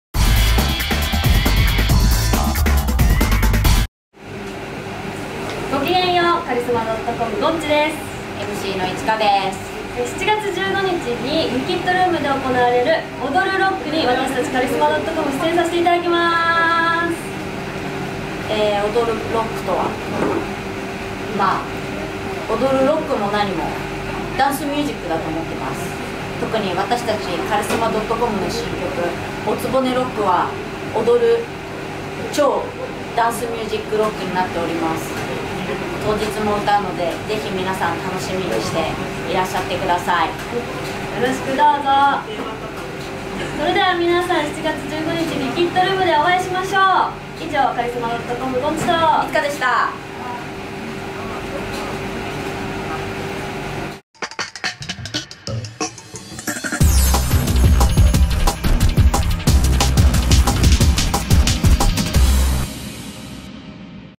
7/15開催「踊るロック」へCharisma.comからコメント到着！！
監修にダイノジ大谷ノブ彦を迎え7/15恵比寿LIQUIDROOMにてTOWER RECORDS presents 踊るロックの開催が決定。
Charisma.comから踊るロックへコメント動画が到着しました！
TOWER RECORDS presents 踊るロック
【開催日時】7月15日（水） OPEN 18時 / START 18時30分
【会場】恵比寿LIQUIDROOM
【出演】BRADIO、Charisma.com、SAKANAMON、MOP of HEAD、DJダイノジ
【チケット】オールスタンディング￥3,000（税込）＋1d